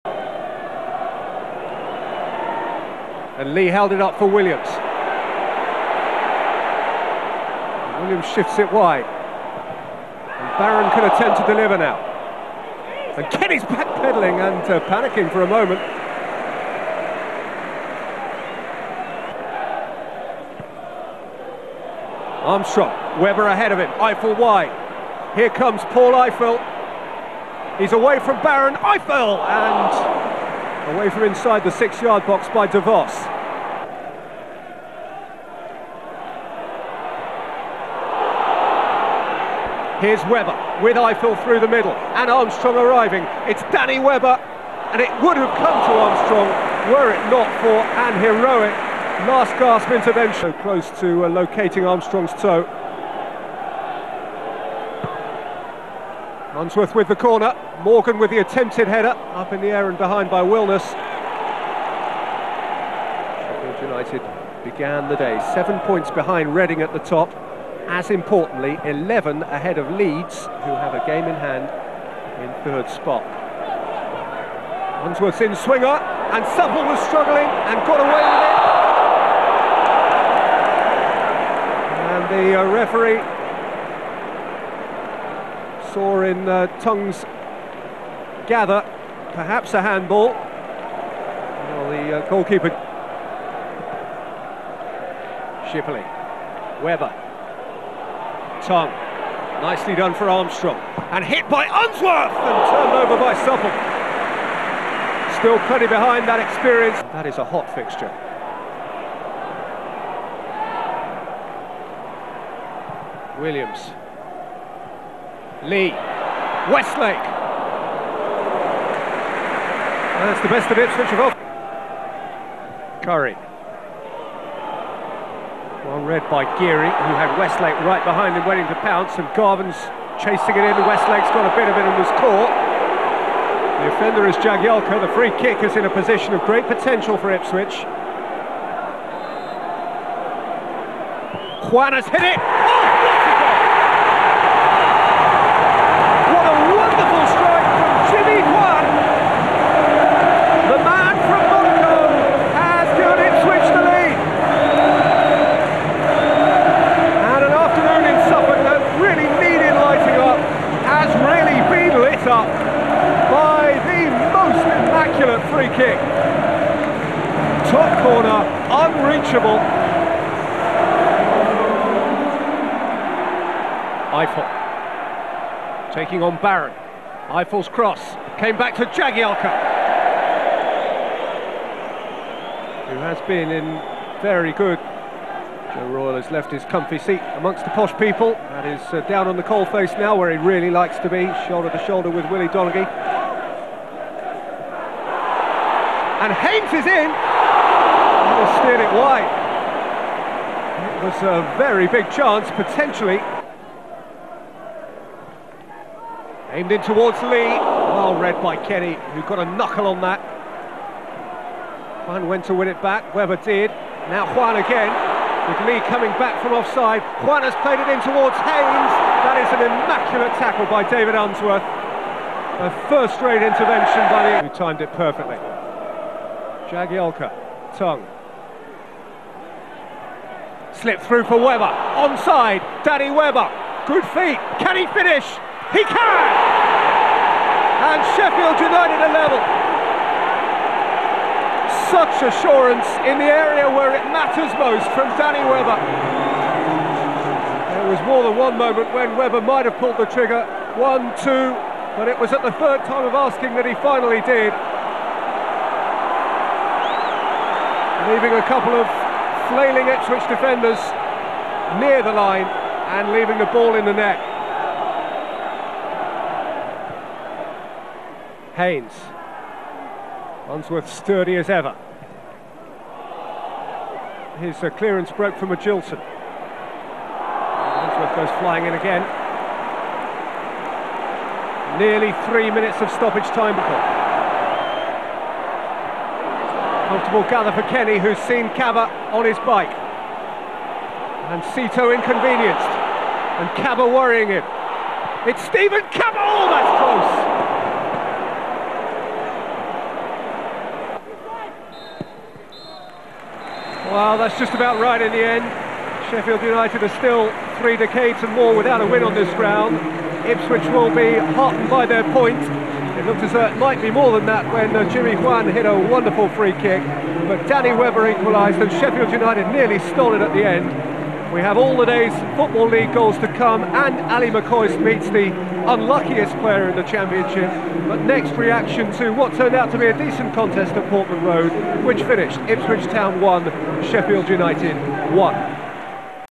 Ipswich Town
Jimmy Juan Scoring a fantastic free kick!